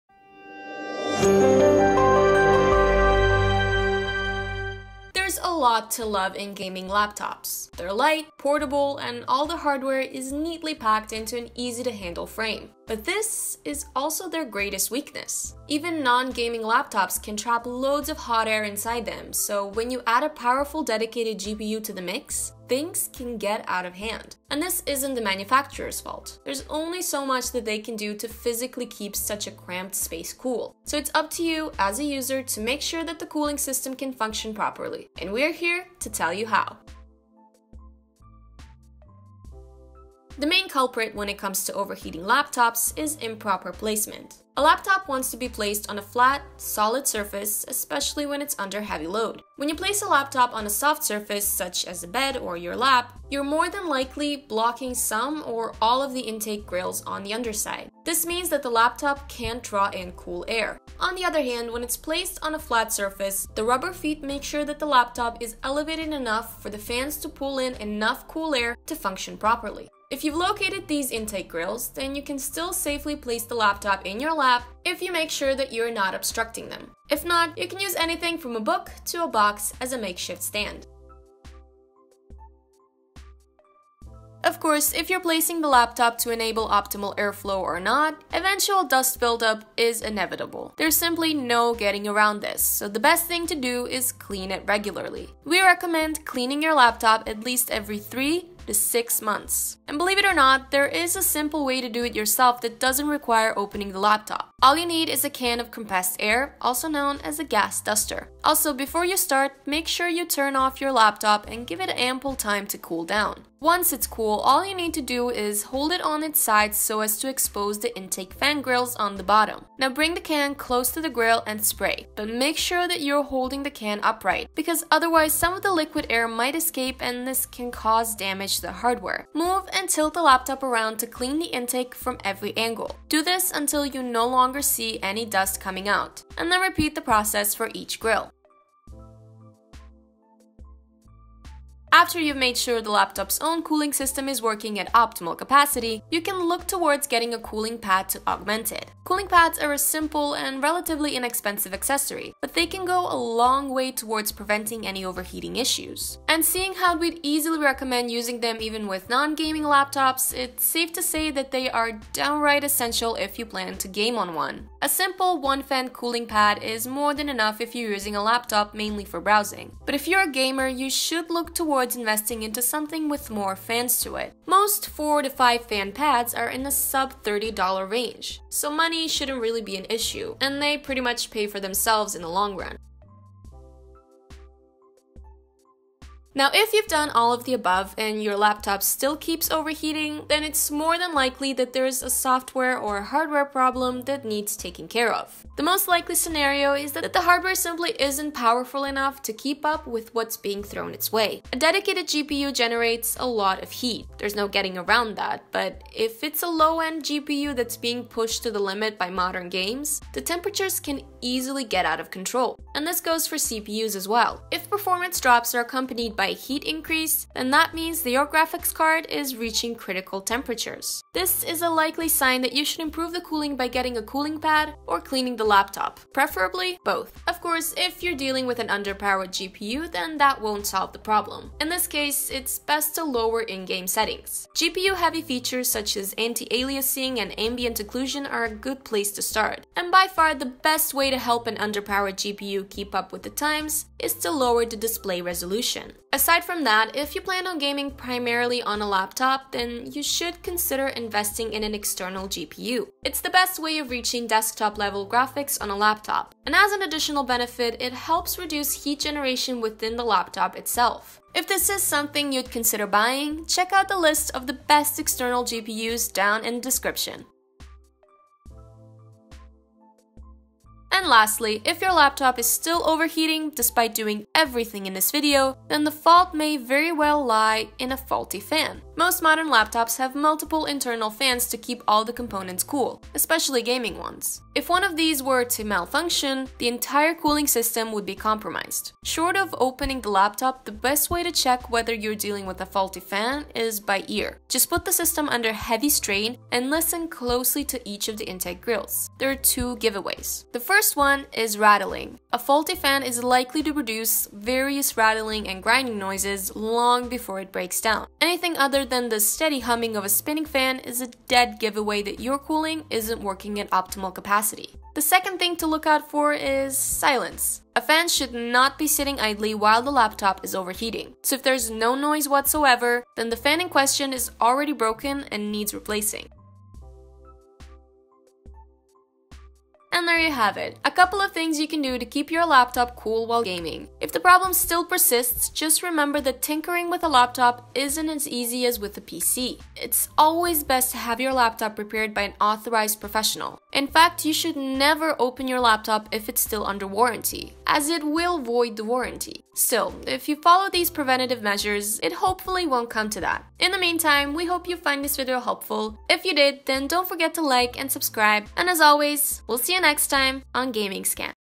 How To Keep Your Laptop Cool While Gaming [Simple Guide]
Does your laptop suffer from high temperatures? Here are some simple things that you can do in order to reduce the heat.

Enjoy a cooler laptop after doing these steps - keep watching!

Timestamps:
0:00 Intro
0:38 Proper Placement
1:22 Cleaning
2:20 Additional Cooling
3:04 Additional Measures: Insufficiently Powerful Hardware
4:28 Additional Measures: Faulty Fans
5:27 Conclusion